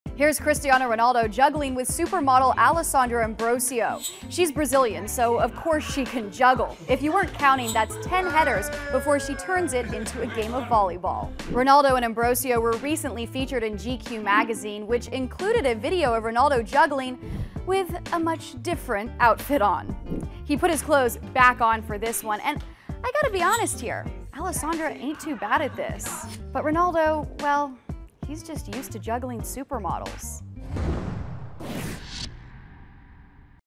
Ronaldo juggles with supermodel Alessandra Ambrosio | FOX SOCCER
Brazilian supermodel Alessandra Ambrosio displays some impressive juggling skills with Cristiano Ronaldo.